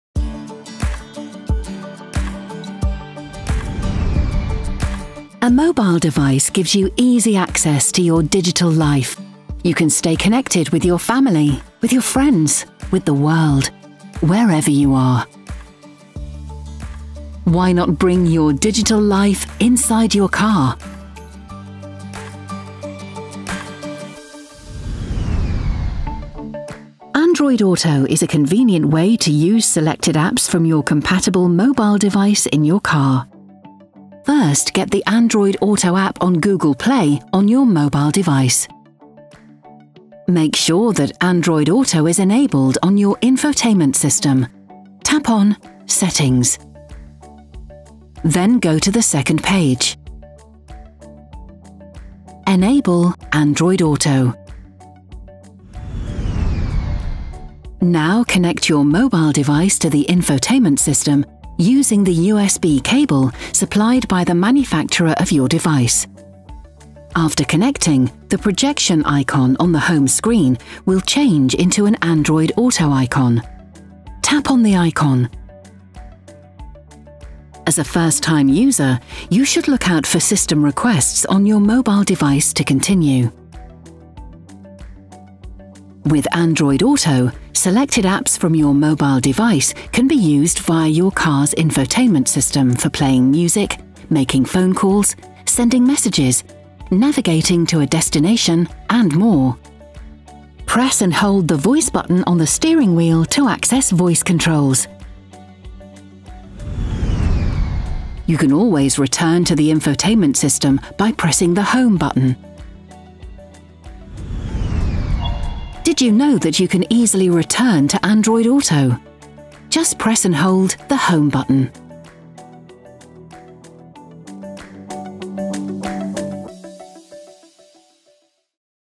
Navi 4.0 IntelliLink | Android Auto™ | That's How It Works!
IntelliLink pairs your smartphone with your car and provides valuable functions for safe and entertaining drives. Here you can quickly find out how to fully operate the Navigation function. In order to unleash the full potential of the system, make sure to check the rest of the explanatory videos – that's how it works!

The instructions in this video are applicable to the following models:
VAUXHALL ADAM (produced as of June 2017)
VAUXHALL CORSA (produced as of June 2017)
VAUXHALL KARL (produced as of July 2017)
VAUXHALL ZAFIRA (produced as of August 2017)